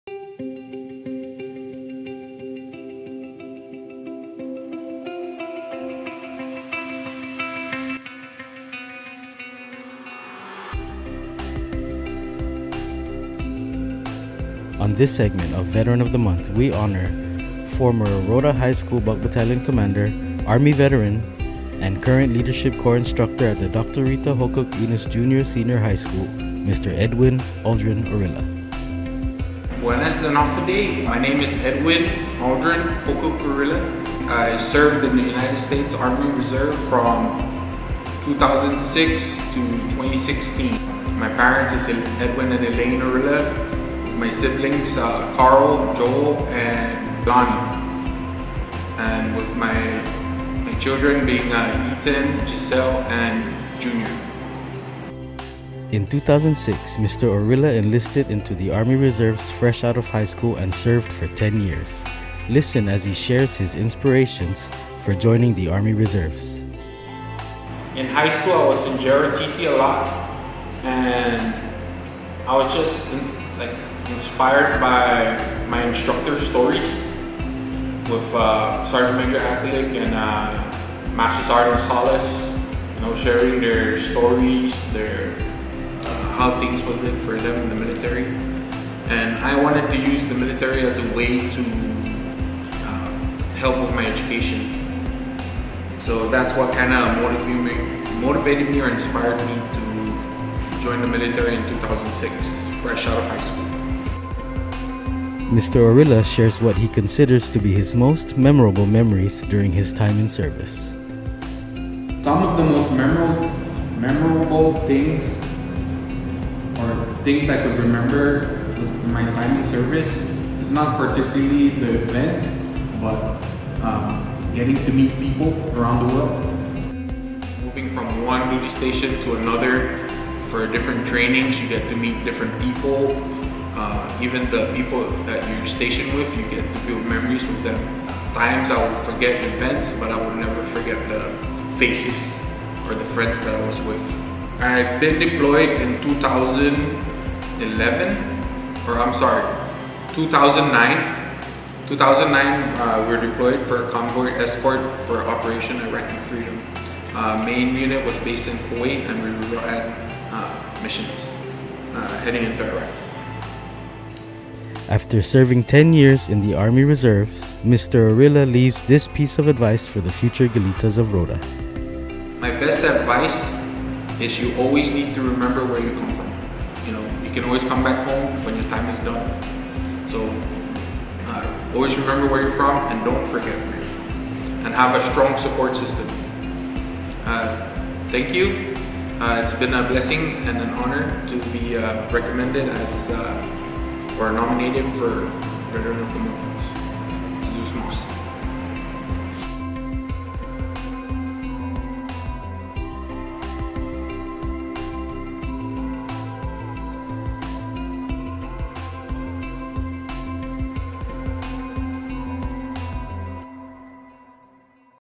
Veteran of the Month #3 [Edwin Aldrin H. Orilla.]
This month's honoree for Veteran of the Month is Mr. Edwin Aldrin H. Orilla. Mr. Orilla  was a former Rota High School (RHS) JROTC Buck Battalion Commander. He is currently the Leadership Corps Instructor at the Dr. Rita Hocog Inos Jr/Sr High School.

Socials:
Facebook: Rota Public Information Office